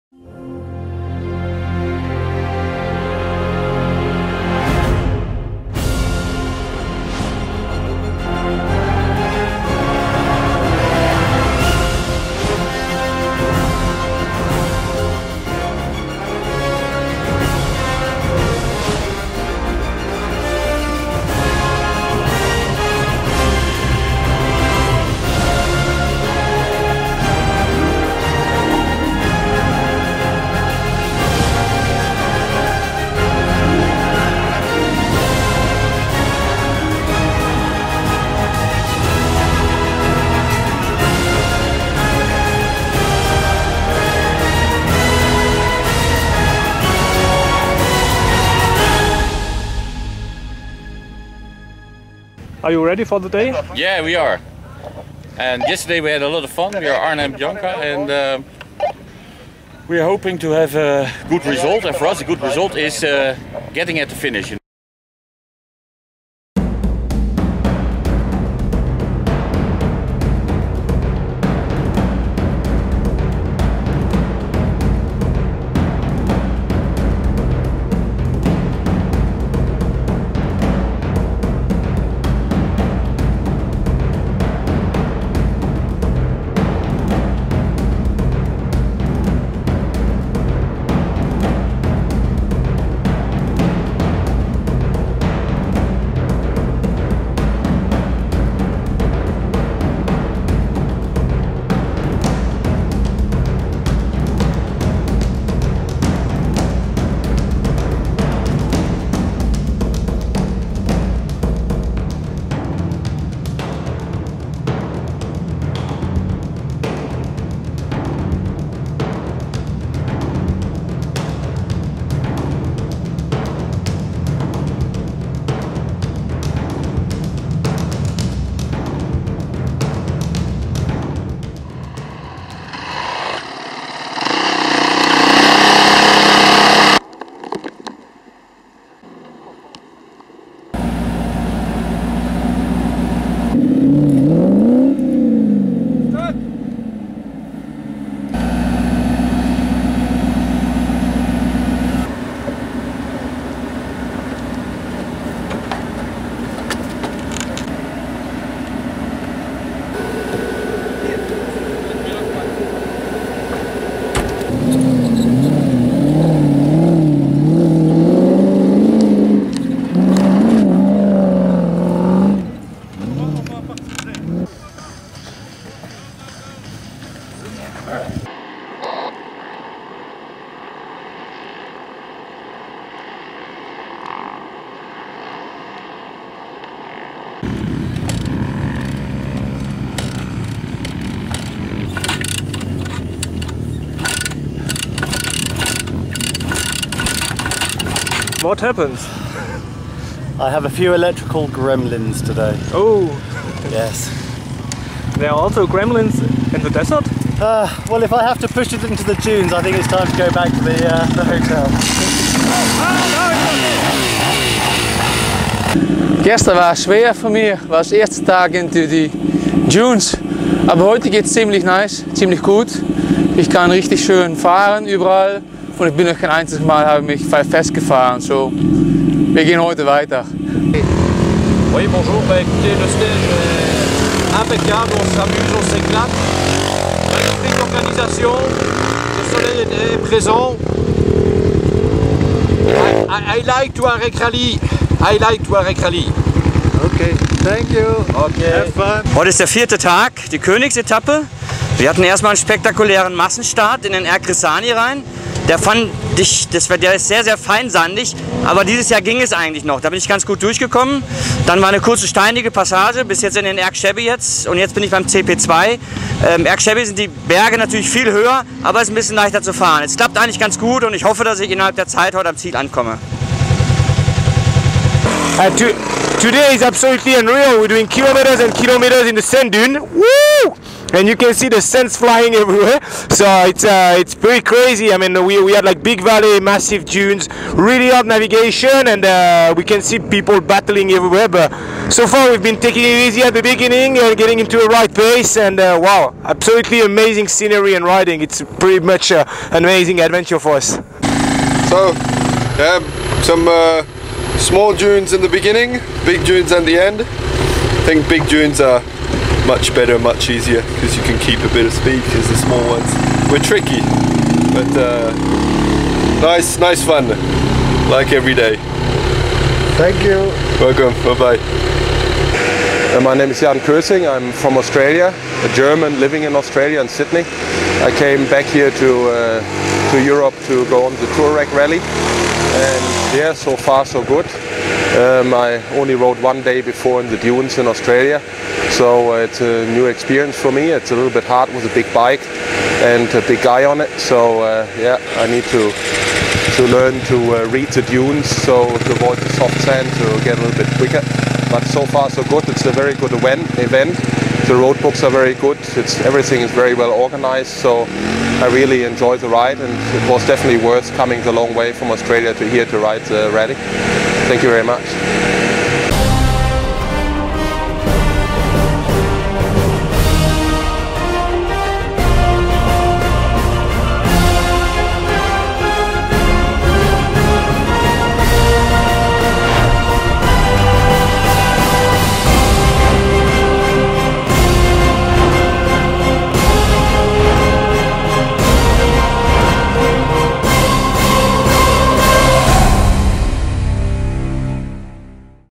Tuareg Rallye 2012 - Day 4 | Kings Stage
Distance:
280 km profis
130 km amateurs
Duration:
9 -- 10 h
Specials:
dunes, dunes, dunes
Camp:
Merzouga